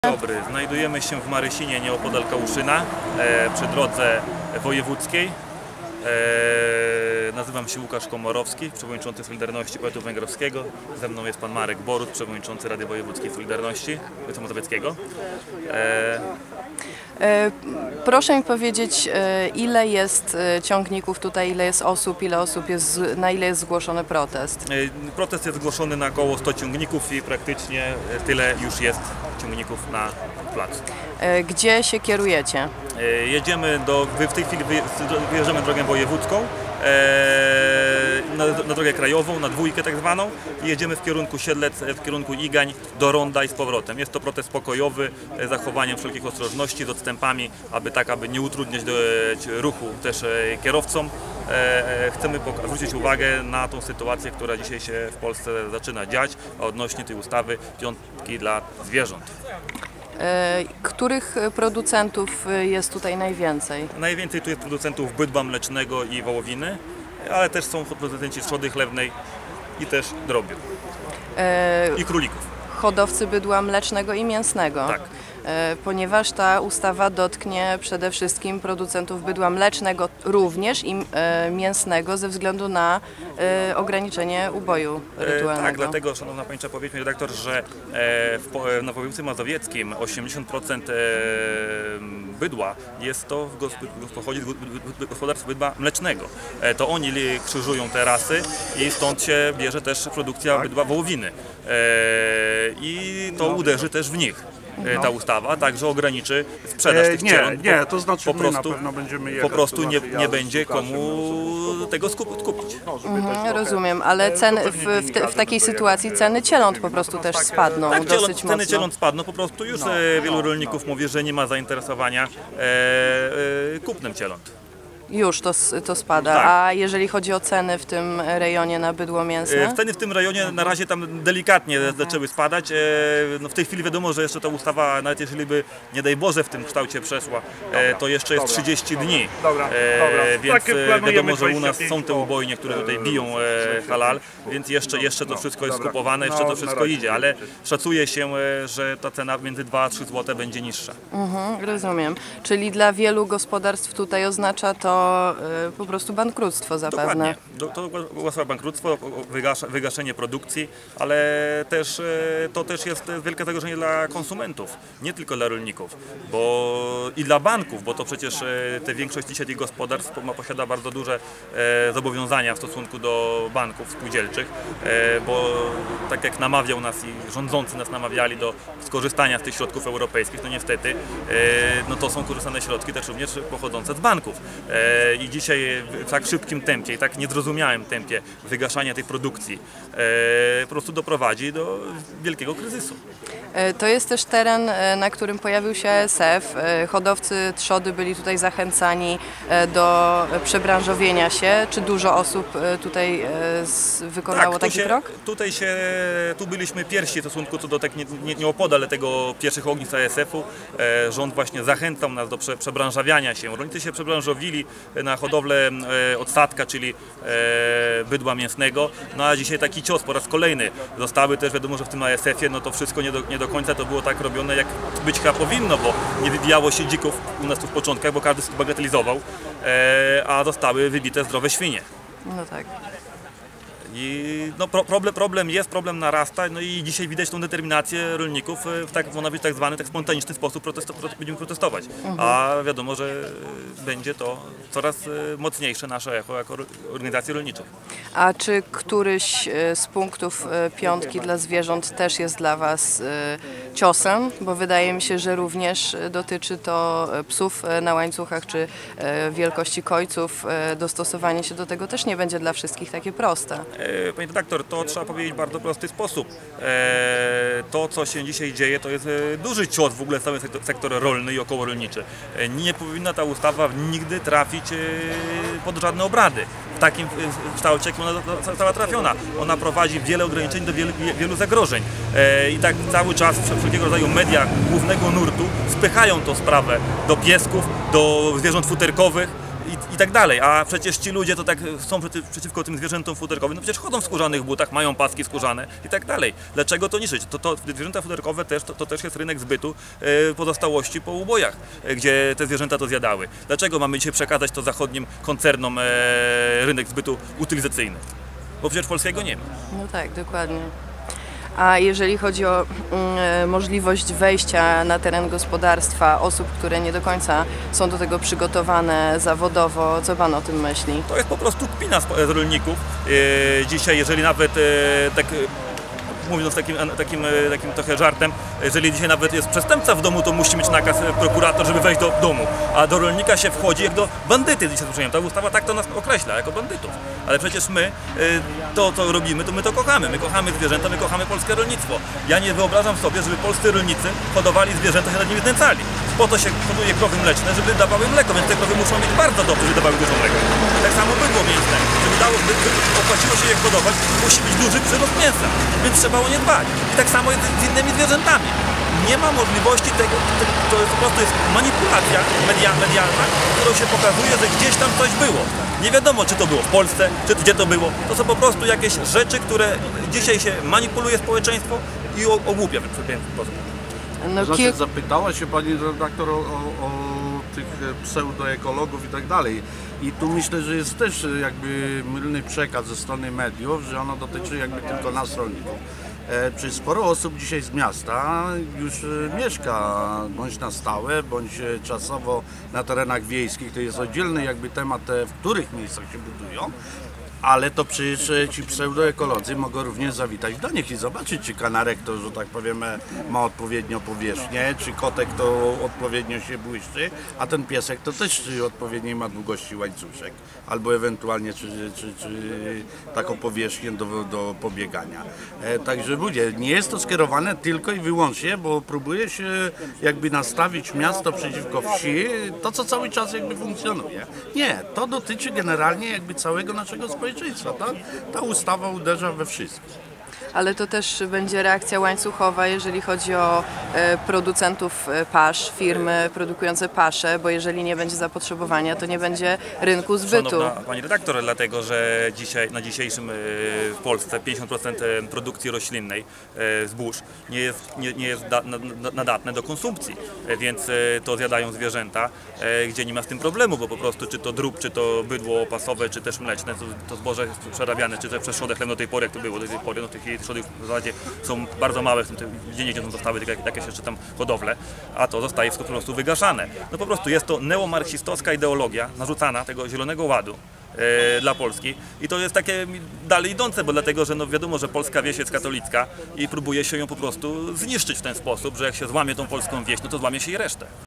Dlaczego rolnicy protestują? Łukasz Komorowski i Marek Boruc o trudnej sytuacji rolników
W województwie mazowieckim w Marysinie k. Kałuszyna o 10 30 ruszył protest rolników przeciw "Piątce dla zwierząt". Kolumna około 100 ciągników poruszała się w kierunku Siedlec, do ronda. Po drodze dołączali kolejni rolnicy! Organizatorem protestu był Łukasz Komorowski.

Jak się zalogować na swoje konto? ↩️

Jeśli jeszcze nie masz prenumeraty sprawdź aktualną ofertę i wybierz pakiet dla siebie ↩️